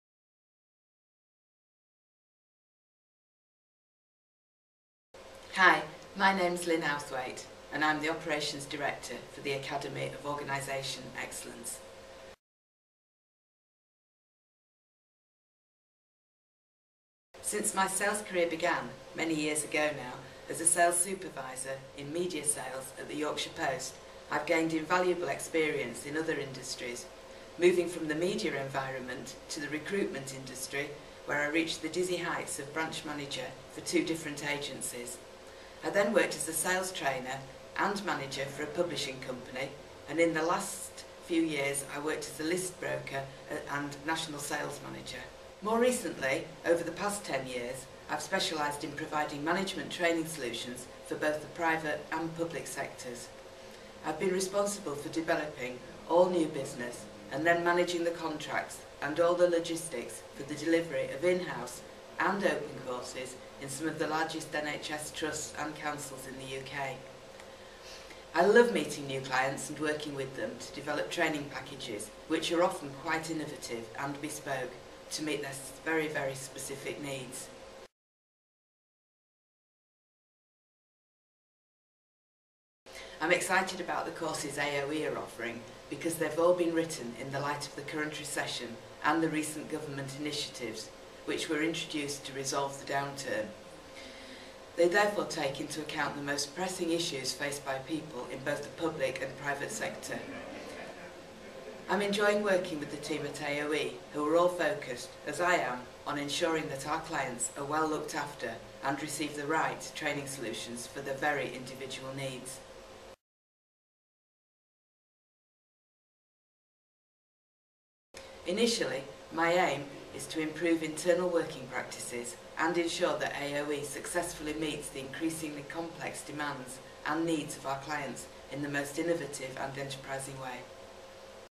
LYNN OUTHWAITE FINAL.wmv
Lynn Outhwaite is an experienced Operations Director, with a successful track record spanning over 25 years, ensuring the organisations she has managed, deliver the very highest quality outcomes for her clients.

Lynn Outhwaite is the Operations Director of the Academy of organisation Excellence Ltd
In recent years, Lynn has specialised in providing Management training solutions for both the public and private sectors, delivering in house and open accredited management training programmes for some of the largest NHS Trusts and Councils in the UK. Working closely with the very best training consultants available, Lynn has enjoyed providing some very innovative and often bespoke solutions for her clients.